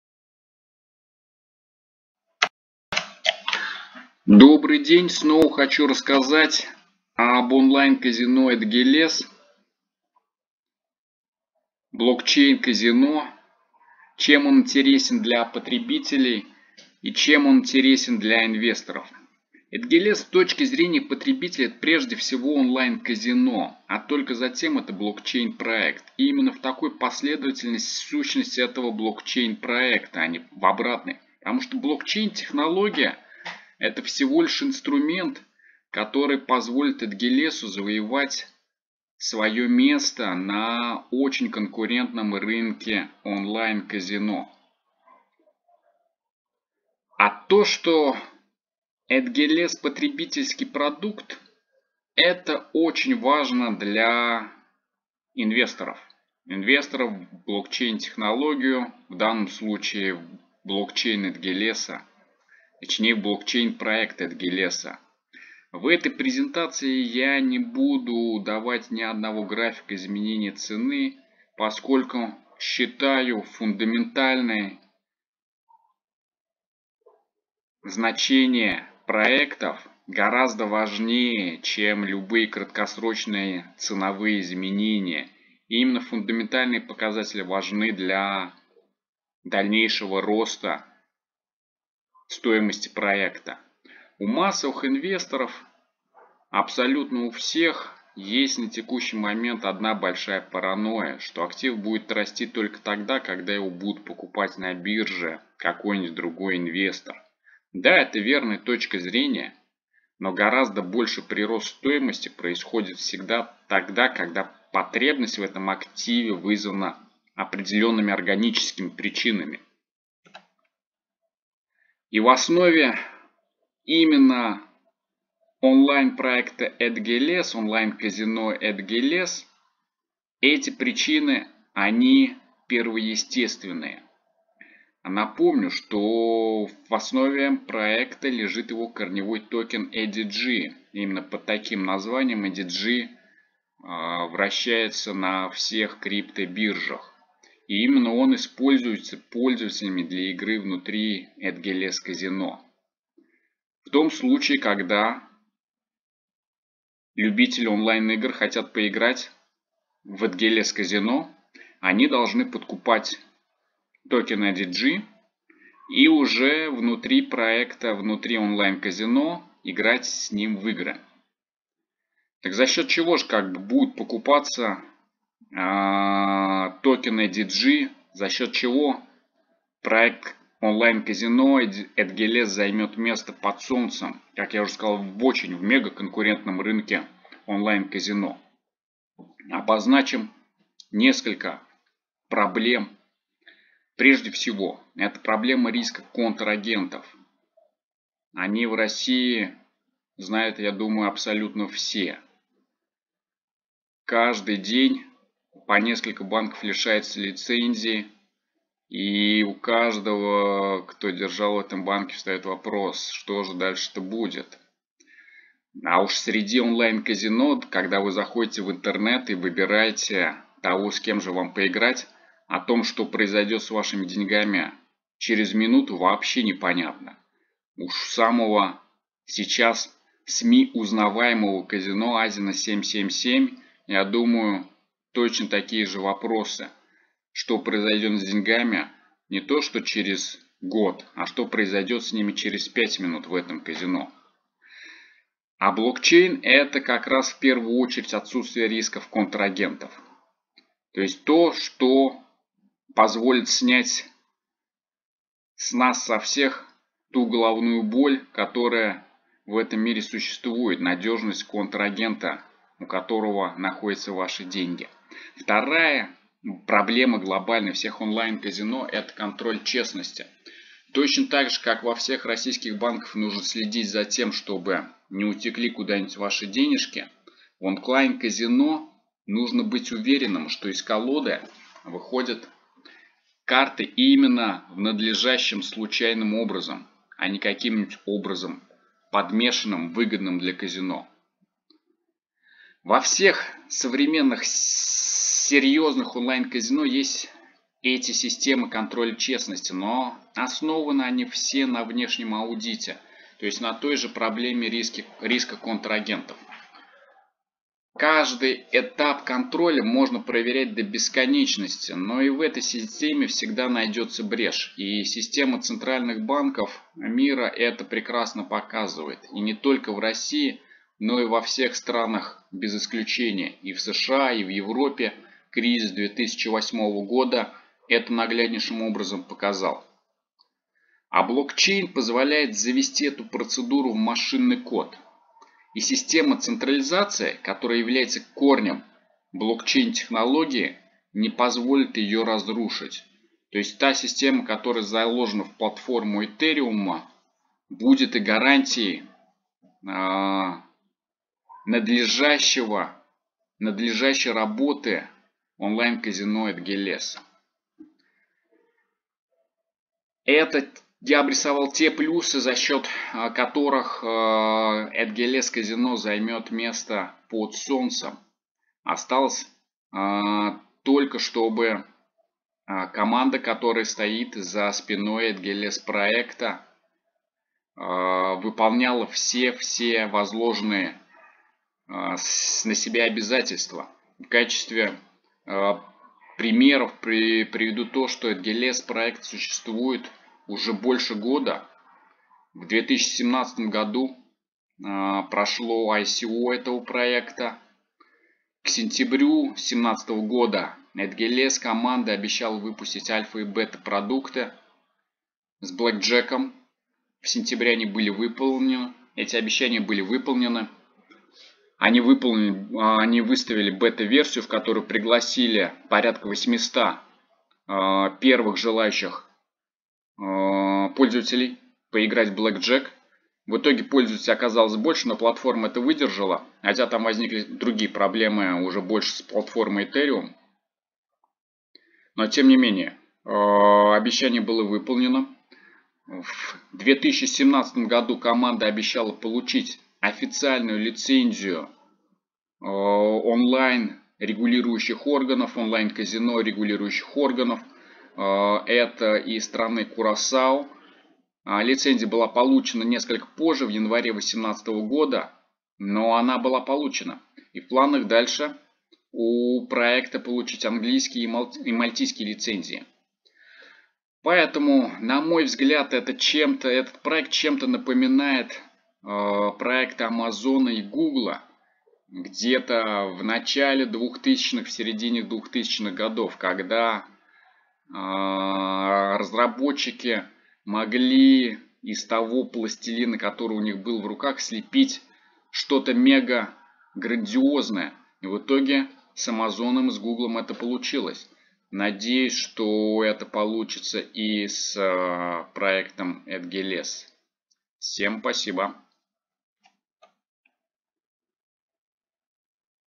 Онлайн Казино Edgeless
Один из моих самых любимых блокчейн проектов. Команда четко выстроила реальный продукт для потребителей на основе технологии блокчейн и смарт контрактов.

Да этим продуктом является Онлайн-казино. Но целью этого видео является не привлечение людей в казино тем более что у нас в России оно запрещено.

А предоставление информации крипто и блокчейн инвесторам о криптовалюте, являющейся неотъемлемой часть этого проекта -EDG. Вложение в EDG сейчас это равнозначно покупке акций Казино Лас-Вегаса и Макао в 60-ые года прошлого века.

Команда реализовала масса реальных вещей , которые приведут к росту стоимости проекта и соответсвенно к росту стоимости криптовалюты EDG.

Первое  - они создали реально работающую игровую платформу . На ресурсе уже 6 популярных азартных игр. Второе - у них есть совершенно официальная лицензия страны Кюрасао. Позволяющая им быть абсолютно легитимной организацией , а не просто IT-командой. Третье - у них четкая бизнес структура, с отделом маркетинга и отделом разработчиков. Это настоящая Компания, ведущая операционную деятельность. Четвёртое- они реализовали те преимущества блокчейна, которые он в себе несёт - безопасность интернета. Соответсвенно у них большие конкурентные преимущества.
Онлайн-казино Edgeless с нулевым преимуществом казино, и имеющее 100% контроль честности. Это главное их конкурентное преимущество над всеми остальными!

- ссылка на русскую группу Edgeless в телеграмме.